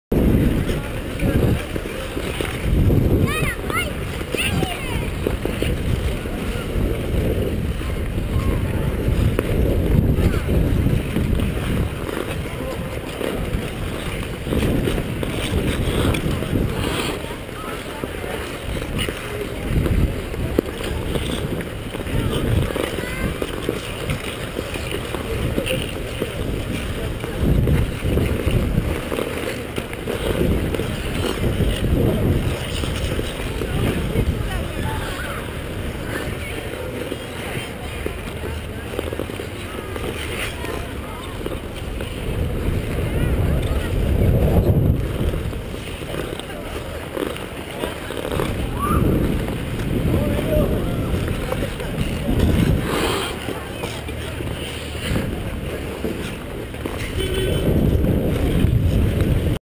Skating at Kitchener City Hall (GoPro test at 1080p)
Just testing out my new GoPro Hero HD by going skating at Kitchener City hall with it strapped to my head.